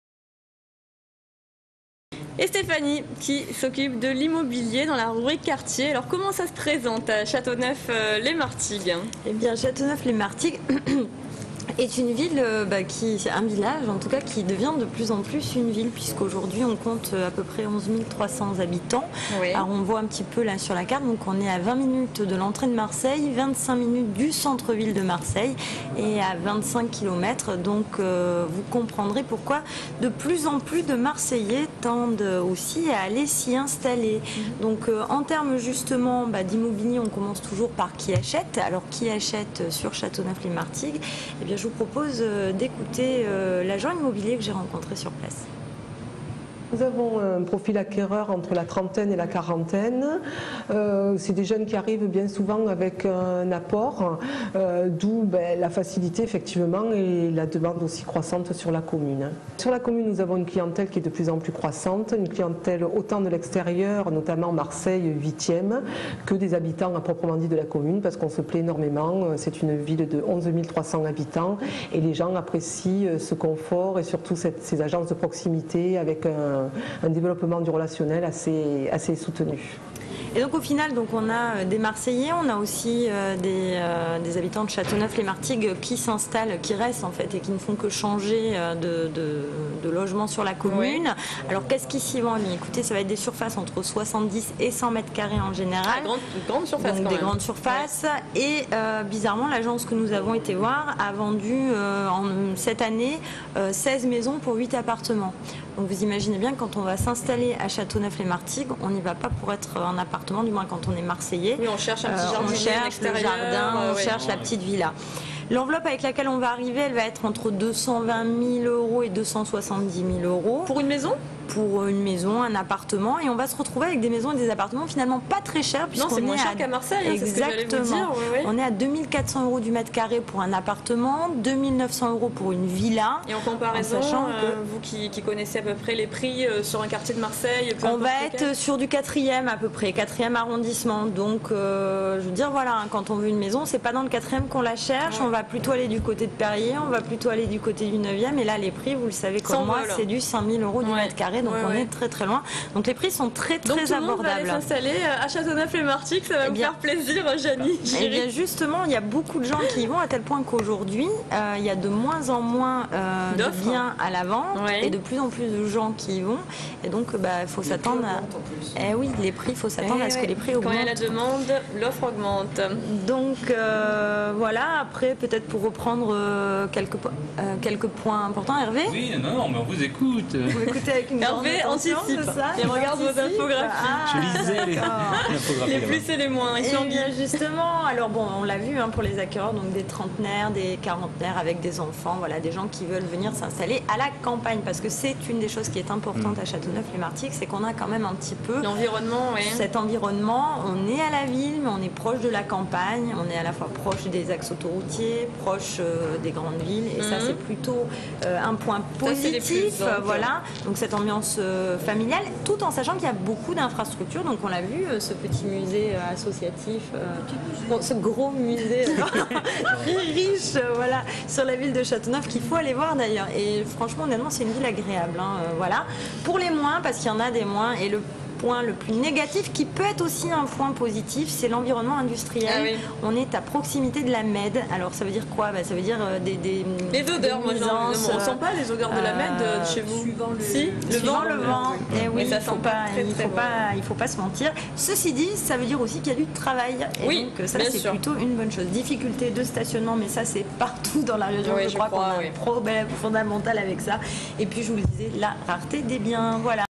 Le marché de l'immobilier à Châteauneuf-les-Martigues
Stéphanie fait le point sur l'immobilier dans le village de Châteauneuf-les-Martigues situé à quelques kilomètres de Marseille. (DROITS RESERVES . Pour toute exploitation sur TV, web, mobile, aller